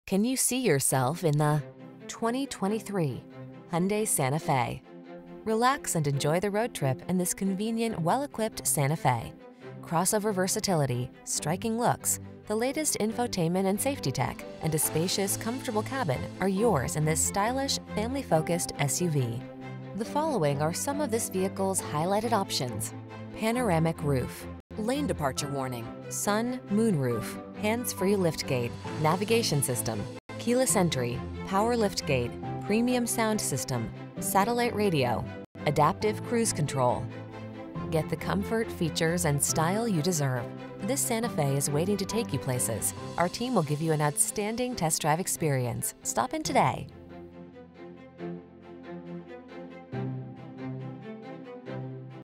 2023 Hyundai Santa Fe Athens, Bogart, Whitehall, Jefferson, Winder, GA 553935
Stormy Sea New 2023 Hyundai Santa Fe Limited available in Athens, Georgia at Sutherlin Hyundai of Athens. Servicing the Bogart, Whitehall, Jefferson, Winder, GA area.

Sutherlin Hyundai of Athens
4160 Atlanta Hwy